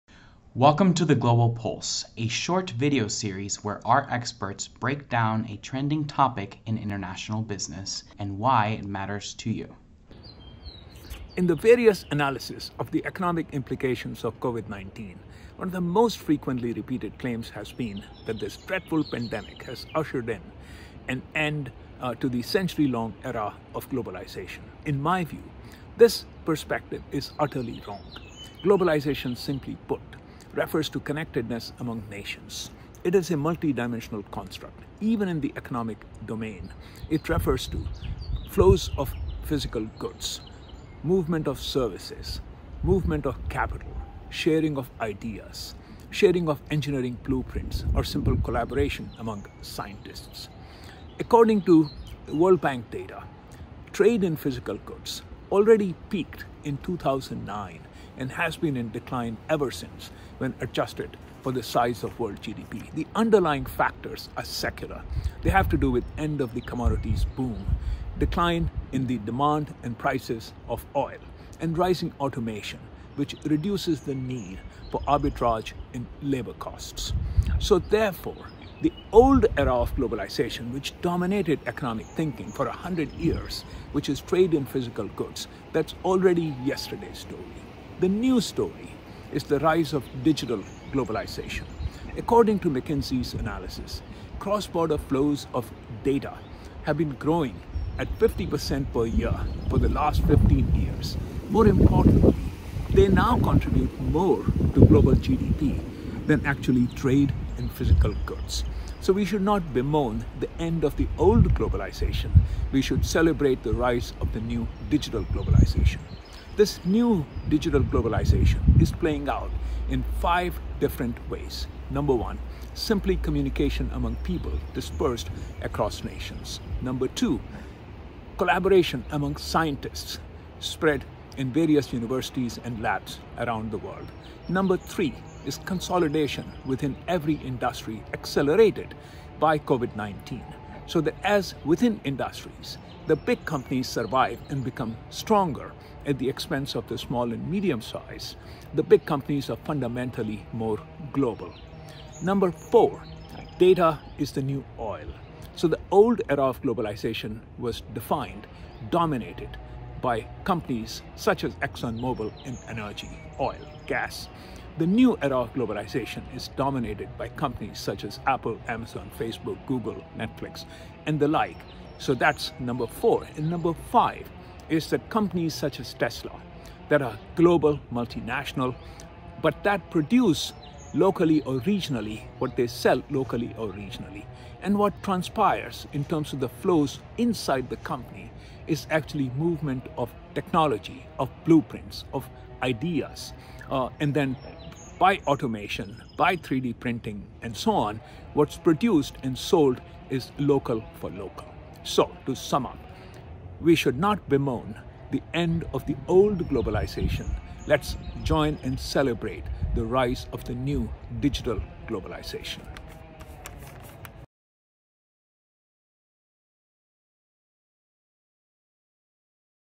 Global Pulse Ep. 23: The New Era of Digital Globalization
Anil K. Gupta, Michael D. Dingman Chair in Strategy and Entrepreneurship, discusses how the world is transitioning out of traditional globalization into the new era of digital globalization.